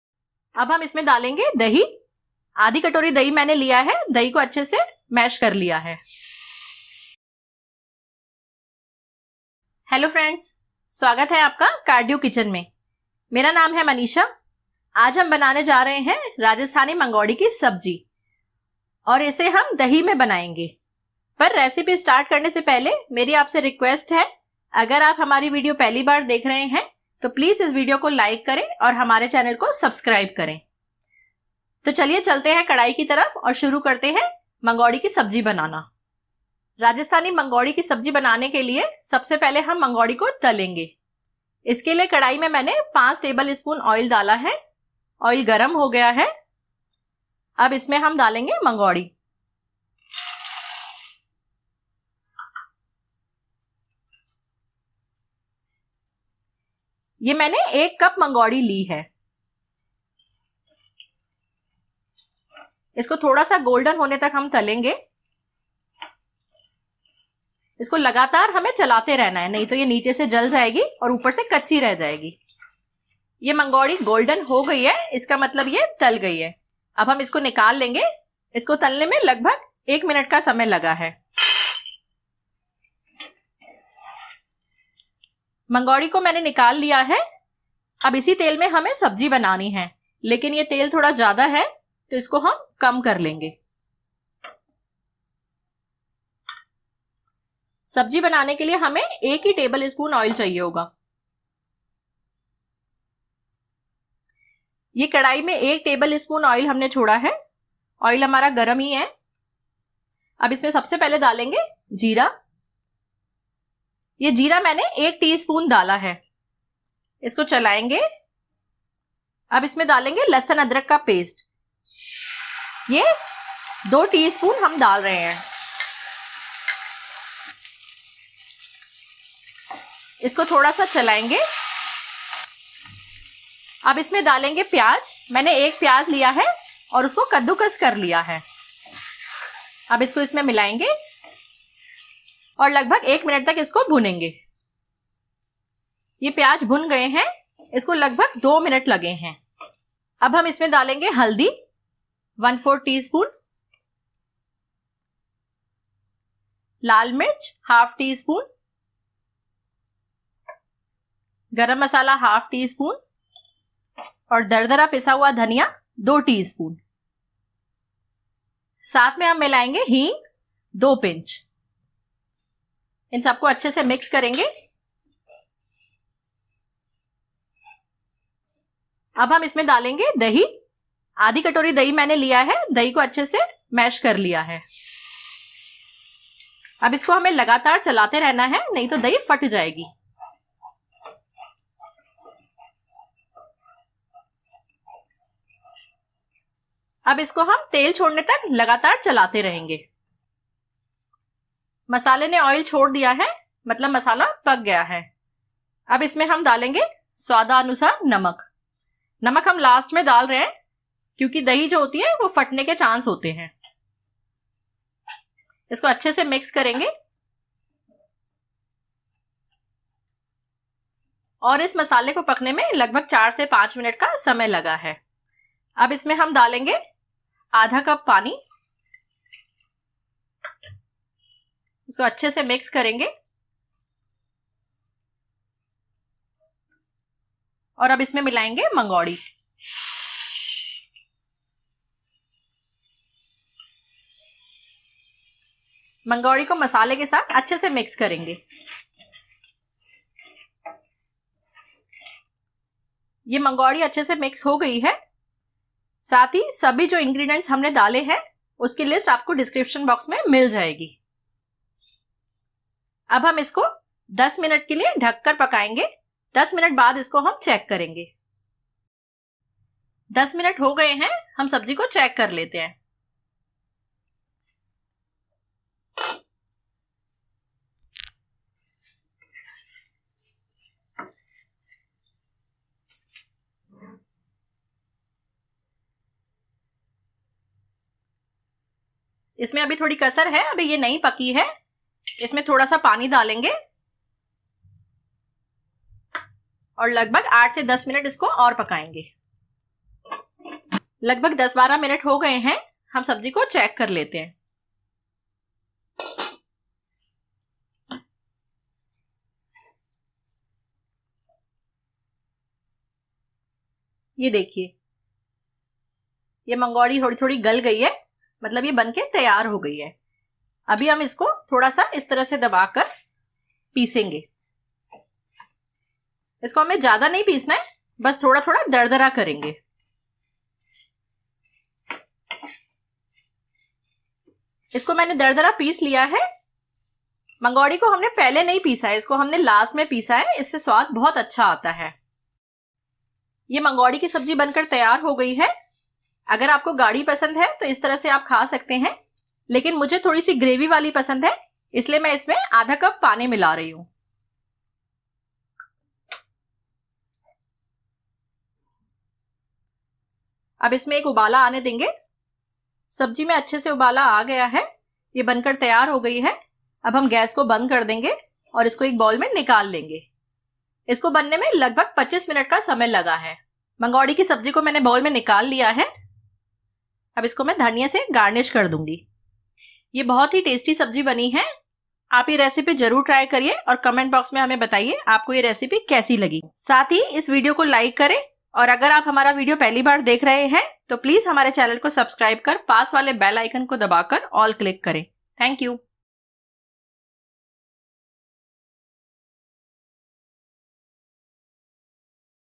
राजस्थानी मंगोड़ी की सब्जी | Dahi Wali Mangodi | Vadi Ki Sabji | Mangodi Ki Sabzi Recipe (Hindi)-2021
राजस्थानी मंगोड़ी की सब्जी | Dahi Wali Mangodi Ki Sabji | Vadi Ki Sabji | Mangodi Curry | Mangodi Ki Sabzi Recipe (Hindi) - 2021

आज हम बनाने जा रहे है बहुत ही टेस्टी रेसिपी जिसको हम दिन में खाने के समय या रात के खाने के समय खा सकते है | बच्चे और बड़े सब इसको शौंक से खाएंगे | यह मंगोड़ी की दही से बनने वाली स्वादिस्ट राजस्थानी सब्जी है | यह राजस्थानी सब्जी बनाना बहुत ही आसान है और इसे आप बहुत आसानी से बना सकते है | इसमें उपयोग होने वाला सामान आप के घर में आसानी से उपलब्ध हो जायेगा | यह रेसिपी आप एक बार जरूर ट्राय किजिएगा |

हमे उम्मीद है आपको यह रेसिपी जरूर पसंद आएगी | आप अपने विचार हमे comment में जरूर बताये |

Ingredients:
Mangodi - 1 cup
Whole coriander (dardara pisa dhaniya) - 2 tea spoon
Curd - 1/2 cup
Ginger garlic paste - 2 tea spoon
Onion - 1 medium
Jeera (cumin seeds) - 1 tea spoon
Garam Masala - 1/2 tea spoon
Oil - 5 table spoon for fry mangodi